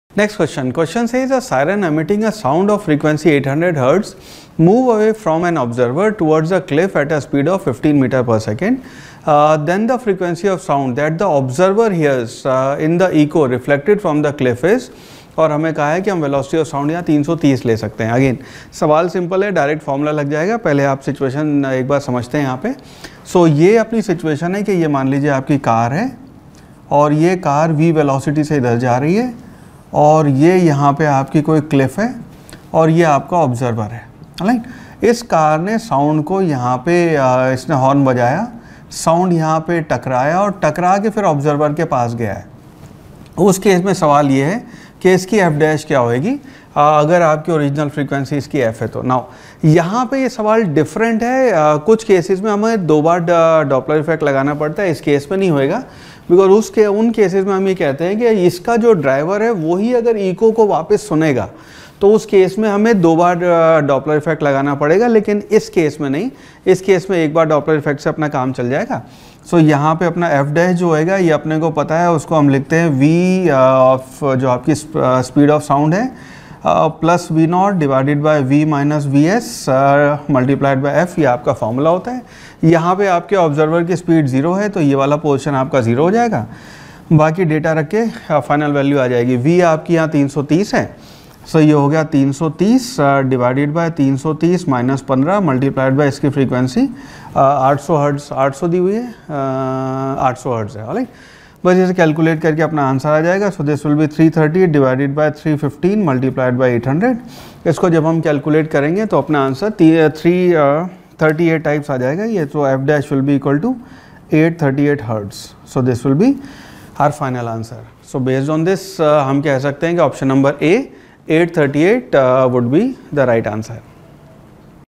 A siren emitting a sound of frequency 800 Hz moves away from an observer towards a cliff at a speed
A siren emitting a sound of frequency 800 Hz moves away from an observer towards a cliff at a speed of 15 m/s.